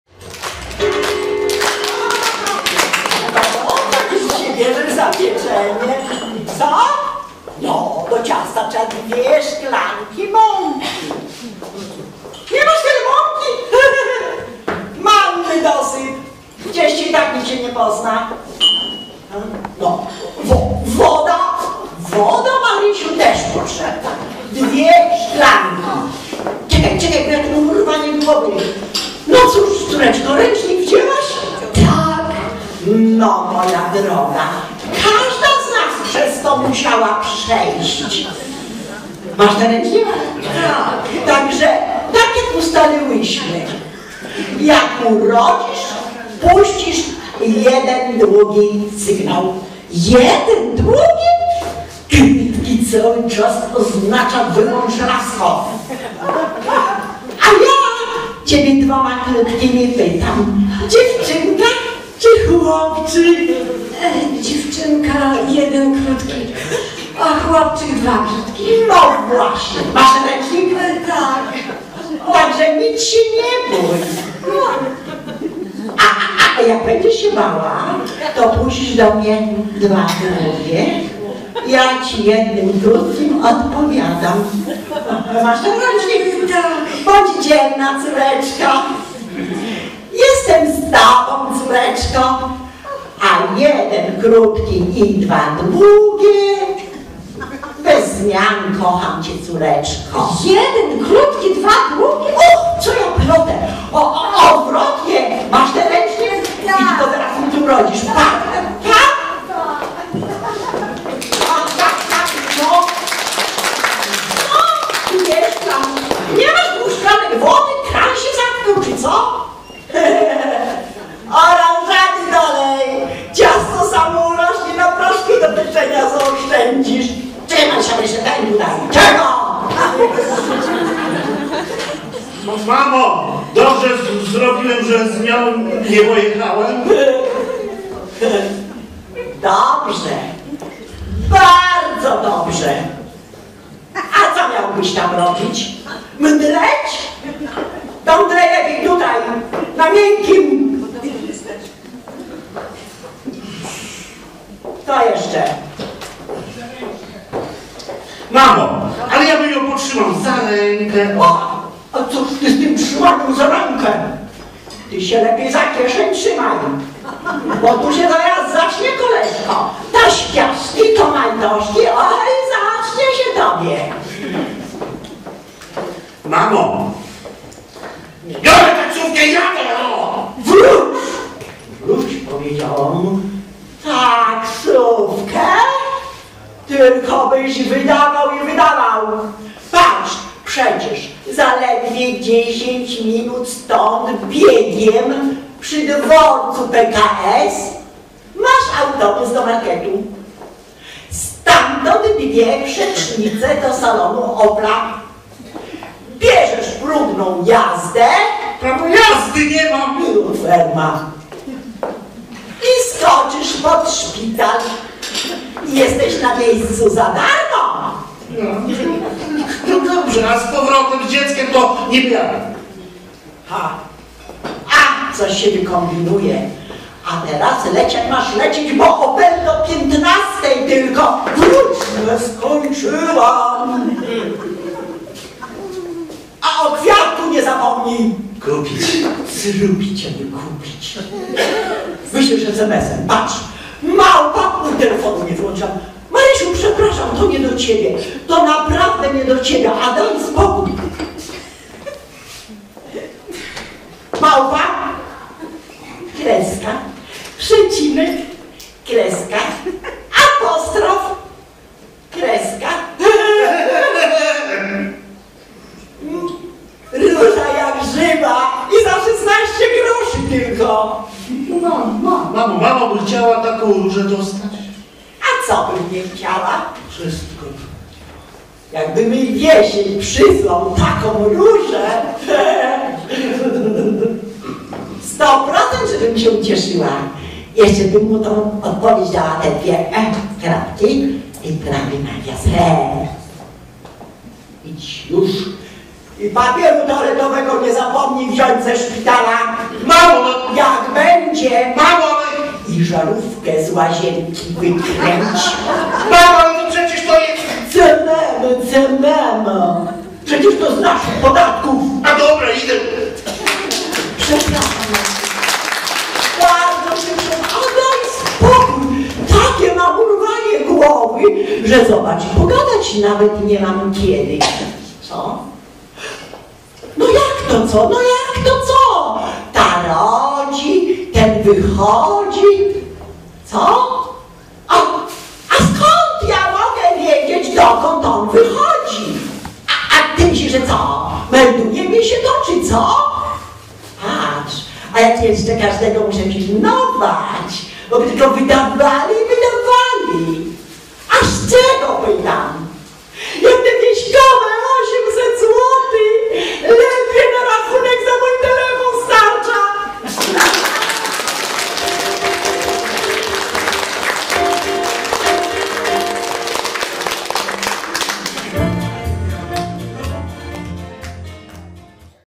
Cz. 03 - skecz - Rodzinka, ach rodzinka - Kabaret LUMBAGO - 1 SENIORIADA - KOKiS w Kolnie.
Kabaret LUMBAGO -1 SENIORIADA - Kolneński Ośrodek Kultury i Sportu - Kolno - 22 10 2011 r. Jeśli chcesz obejrzeć więcej podobnych filmów, to kliknij w ten